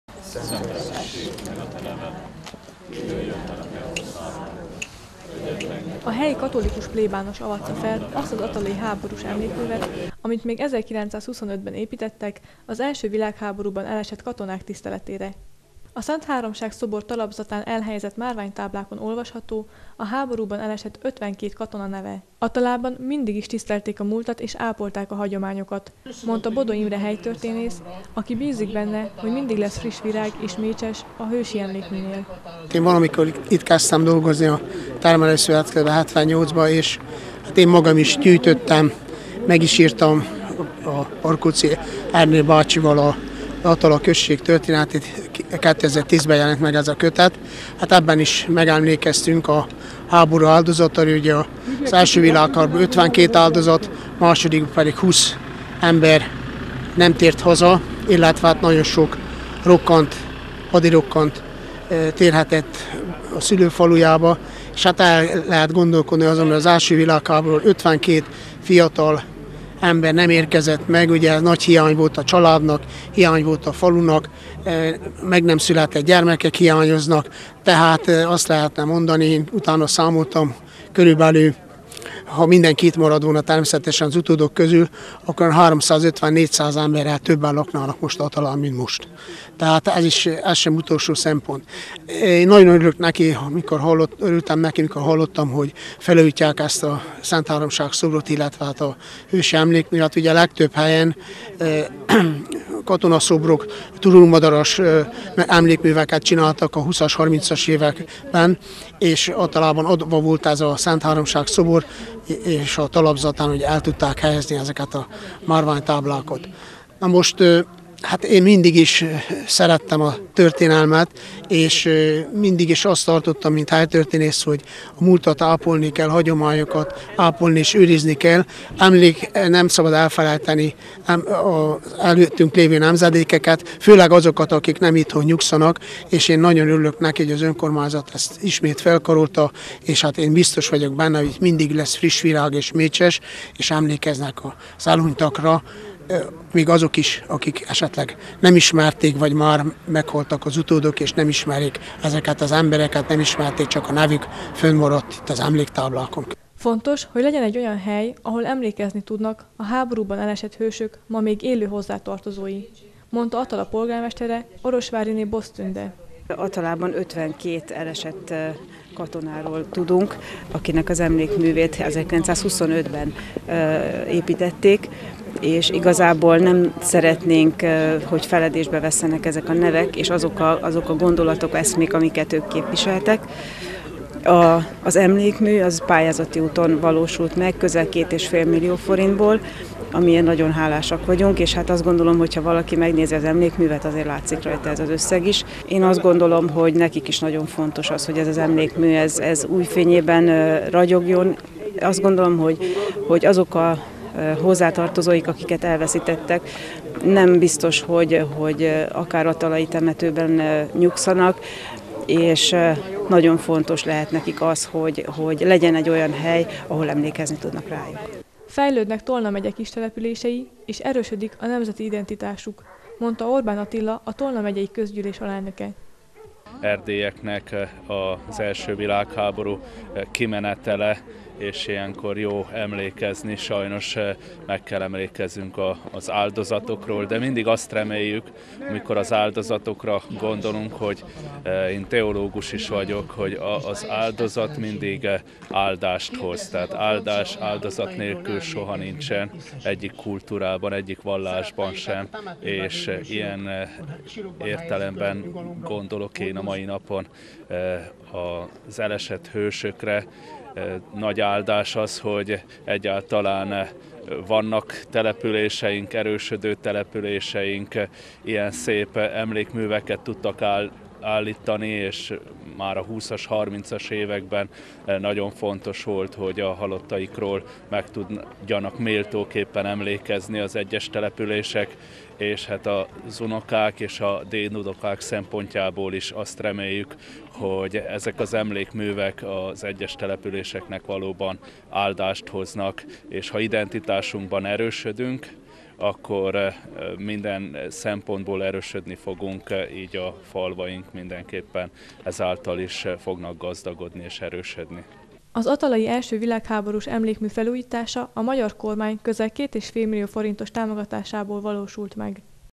Felújították Attalán az első világháborús emlékművet. 20191203
Felújították Attalán az első világháborús emlékművet. A szobor talapzatán olvasható annak az 52 attalai katonának a neve, akik életüket adták a hazáért 1914 és 18 között. A felújítás a magyar kormány közel 2 és fél millió forintos támogatásával valósult meg.Tolnatáj TV- Tolnatáj Televízió